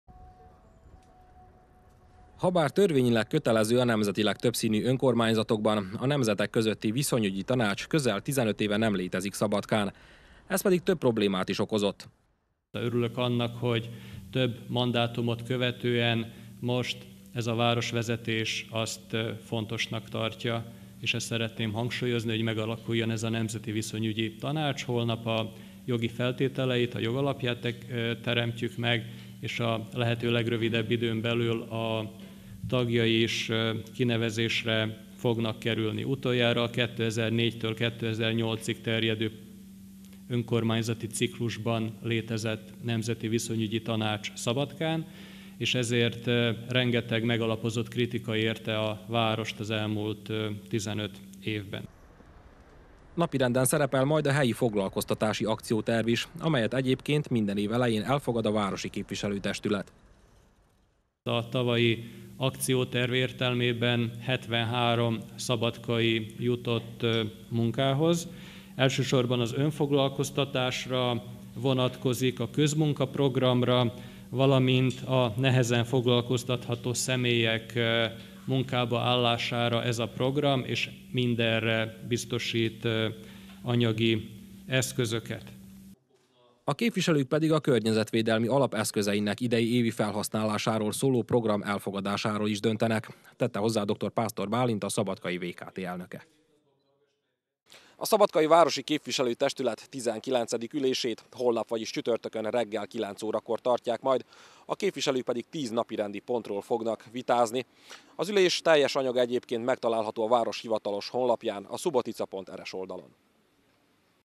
2022.02.02. - Ismét létrejöhet a nemzetek közötti viszonyügyi tanács Szabadkán
A VKT holnapi ülésén erről is döntenek majd a képviselők.